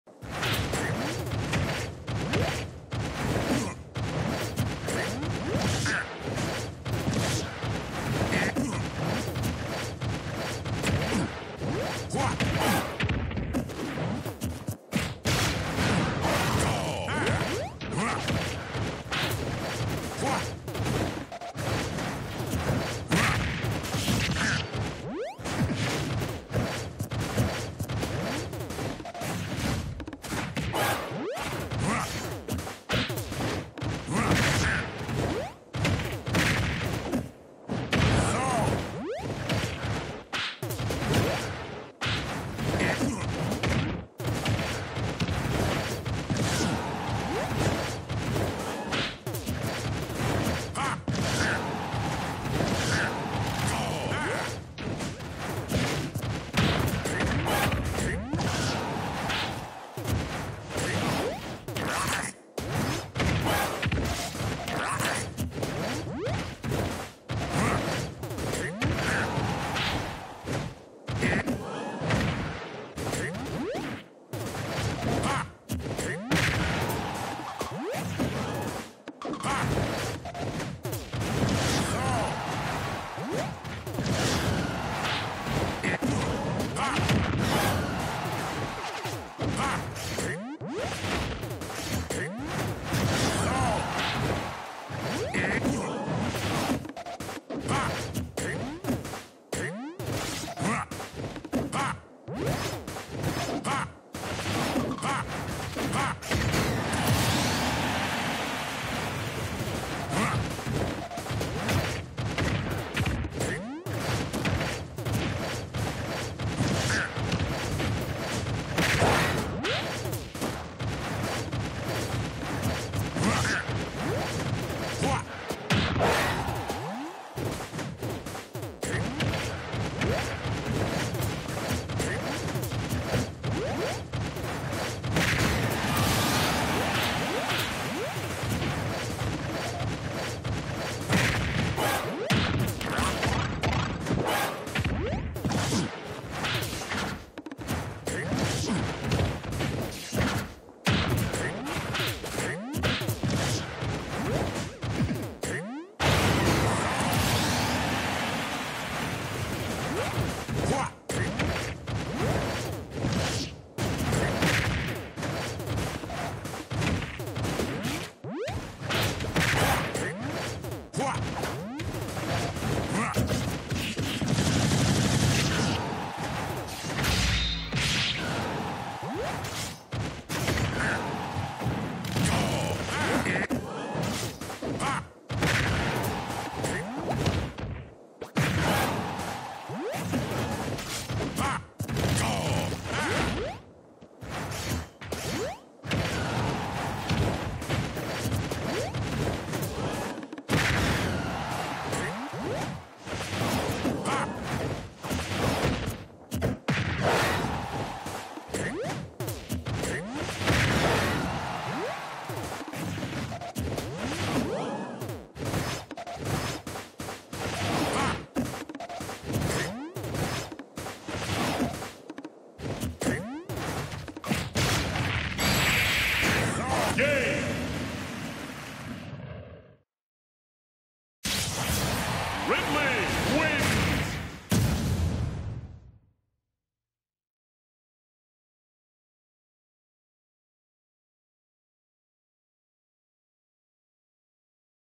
The Greatest Ridley Gameplay
"I'm gonna hold forward and press nair and you're going to like it" -me trying to get in last minute mechanics practice for a tourney while also being too tired to properly play neutral. Imagine how fast I could nair offline!!

1:40, 2:32, 2:52, 3:34

Also the game after this one I lost because my fingers got too tired from mashing nair so much